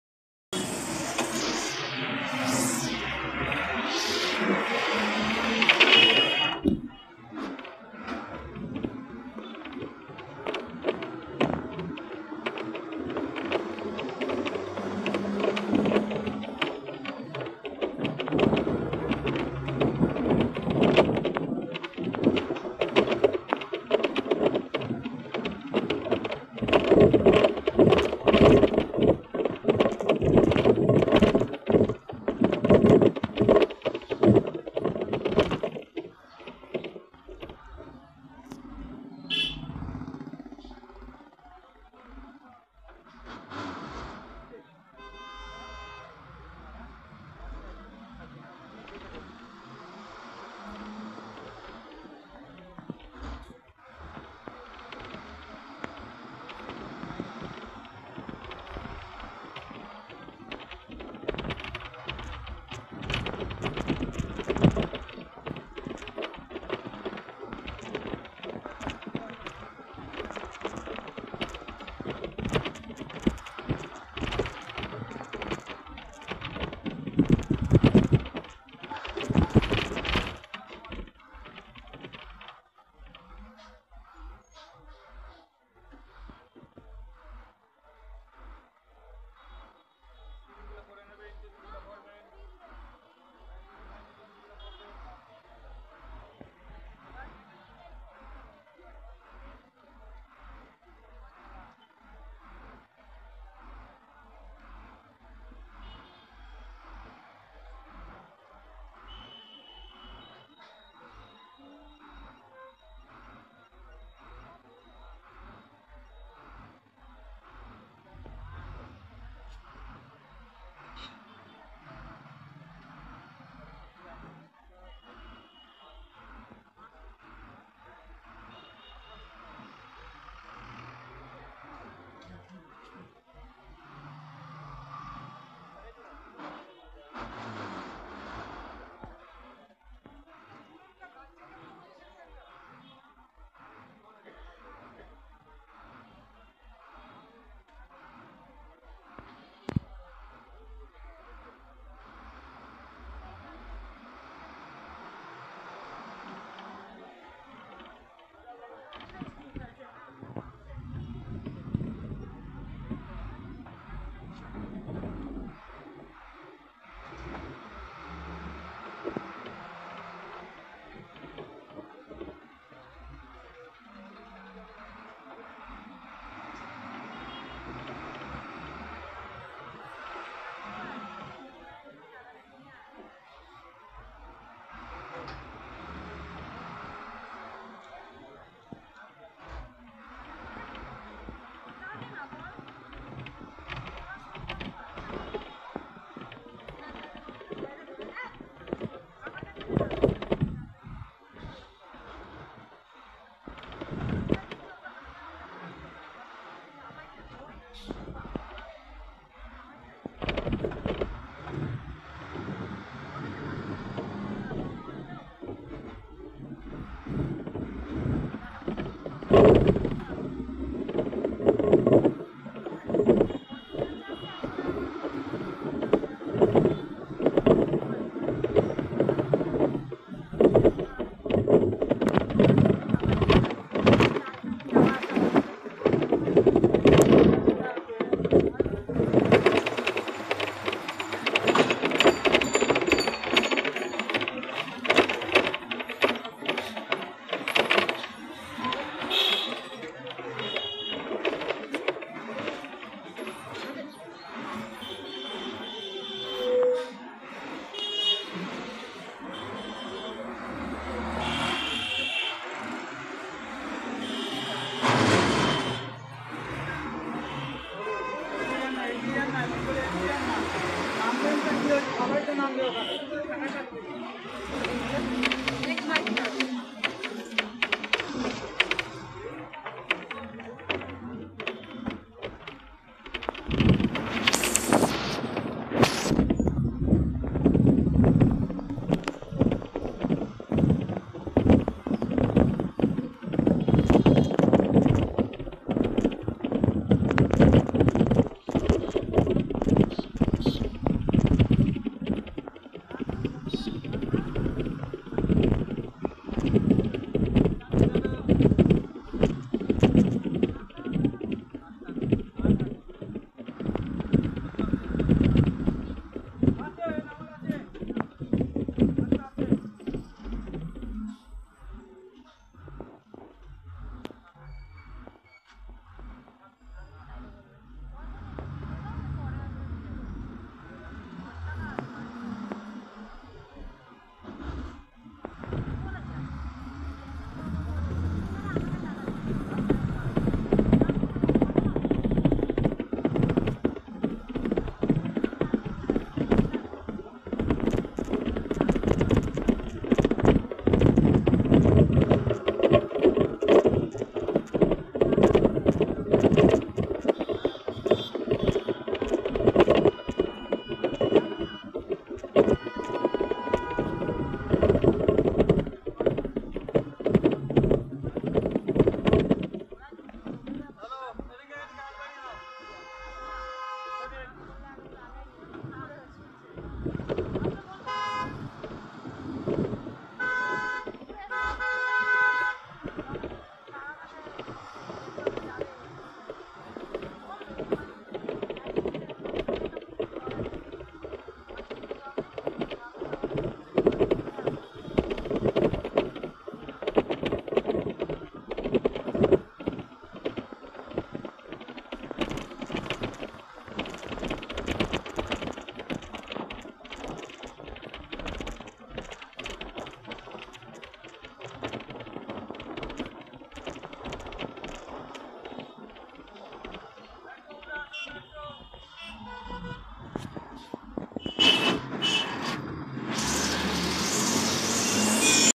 Kolkata Road Guide: Muchipara to Tollygunge
This is kind of Road Guide for Kolkata visitors. This video is made at the beginning of 2017, at the time when many new projects are undertaken in Kolkata like East West Metro , construction of new flyovers and beautification of City under the leadership of Bengal Chief Minister Mamata Banerjee. We believe Kolkata will look prettier in coming years if this kind of work continues. But, basically the roads between places will remain the same. Hence the video just helps you to find how to go between places in Kolkata. And all the best to our Bengal Government for such an effort!
This road covers the following places….